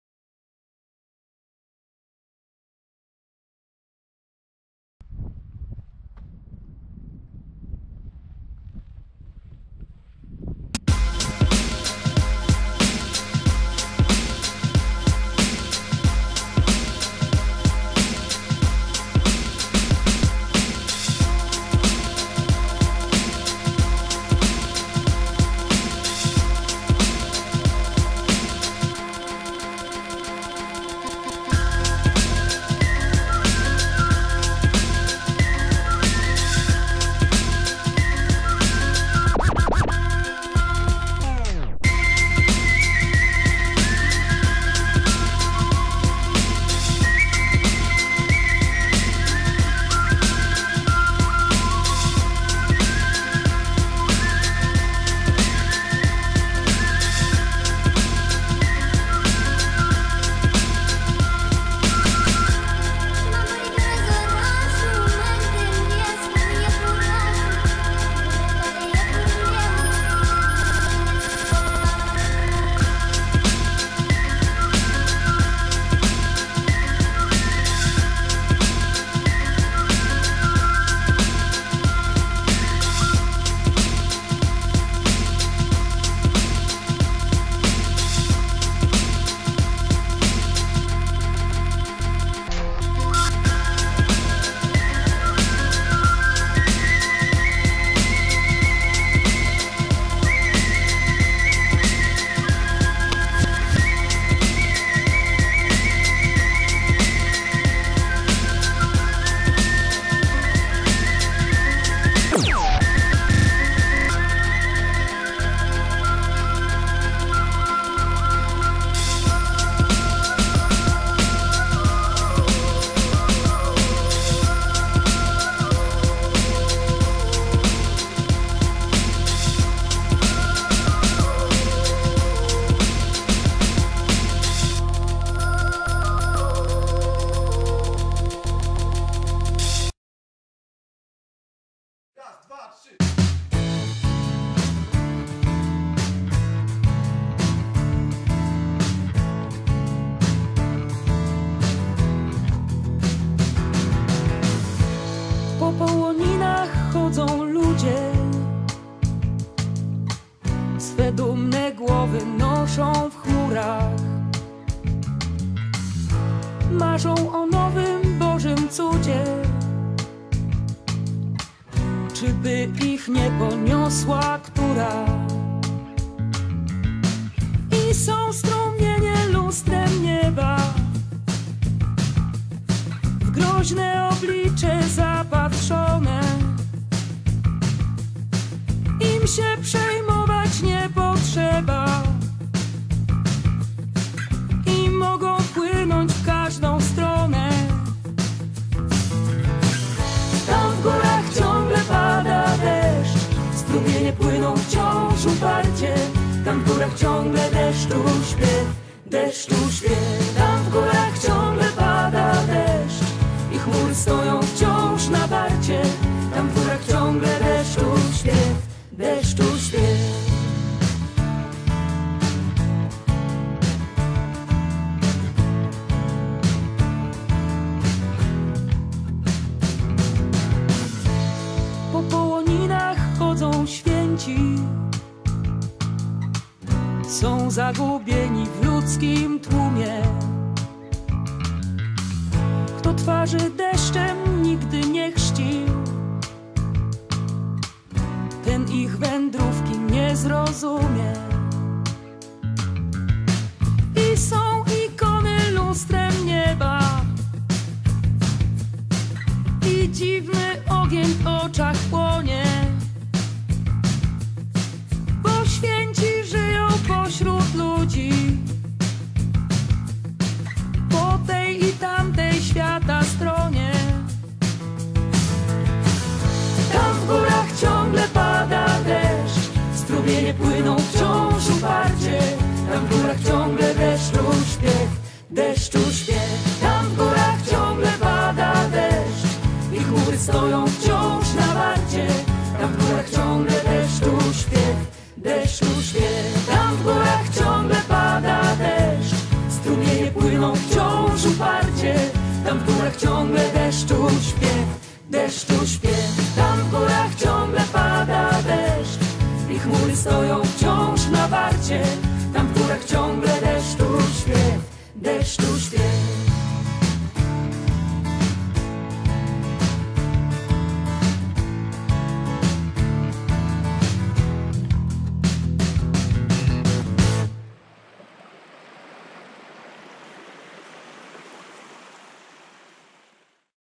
ZIELONE PIENINY: DURBASZKA- WYSOKI WIERCH- WYSOKA- WĄWÓZ HOMOLE, CZYLI SŁOŃCE I DESZCZ.
Wiosna w Pieninach i ta zieleń...czy może być coś lepszego? Przekonajcie się sami.